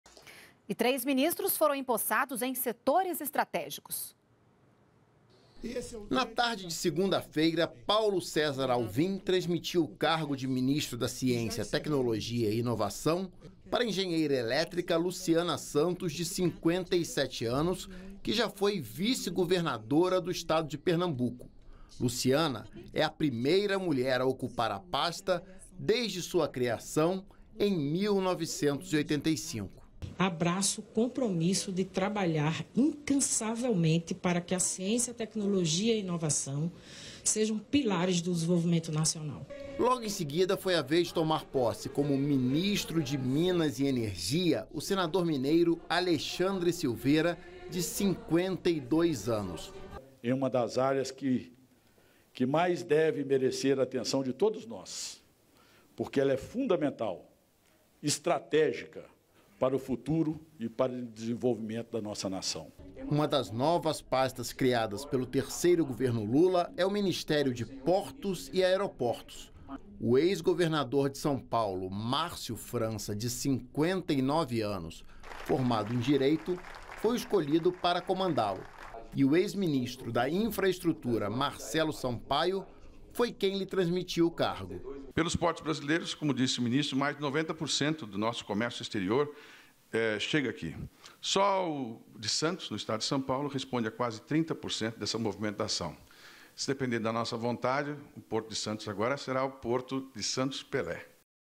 Governo | Novos ministros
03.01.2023 - Luciana Santos assume a Ciência e Tecnologia; Alexandre Silveira, o Ministério de Minas e Energia; e Márcio França, o Ministério de Portos e Aeroportos.